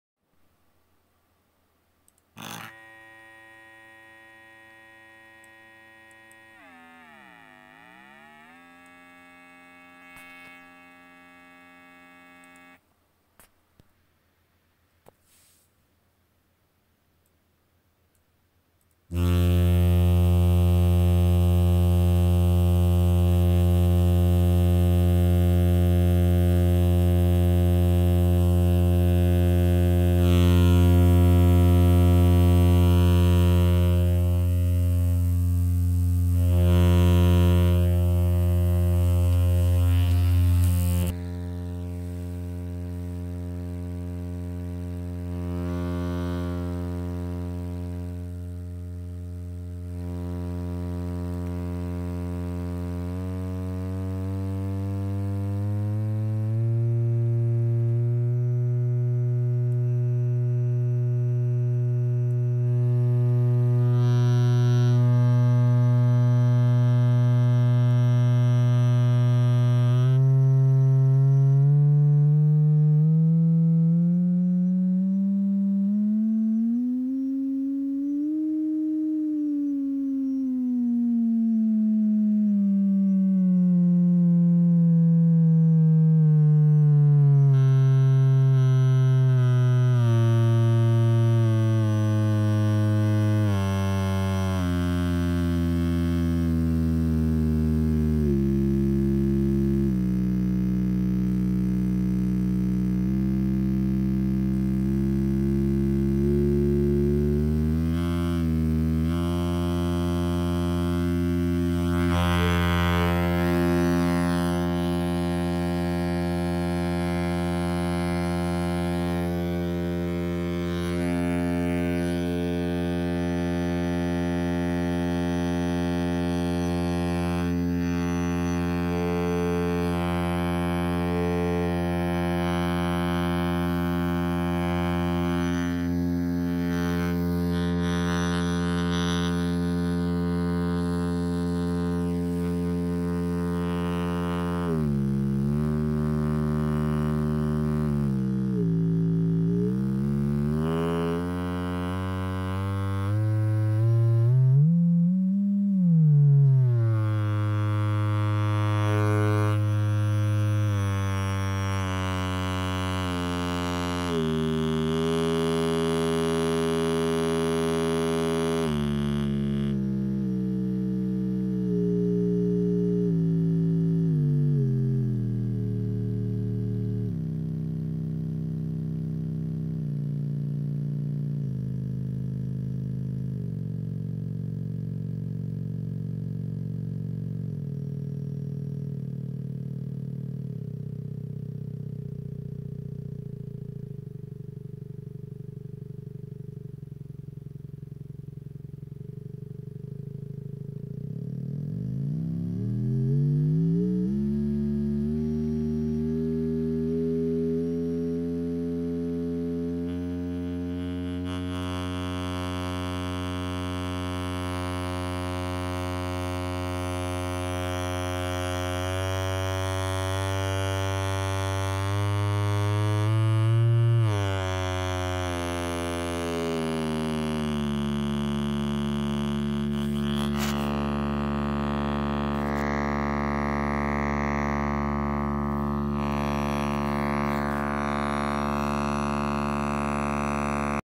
Audio vibration test on calcium carbonate toothpaste suspension
Using a bass exciter on the pc (with an amp board), with a web-based tone generator, to loosen up a suspension. The calcium carbonate settles out and hardens, but I noticed our electric (sonicare) toothbrush vibrates right through it, so I wanted to test vibration...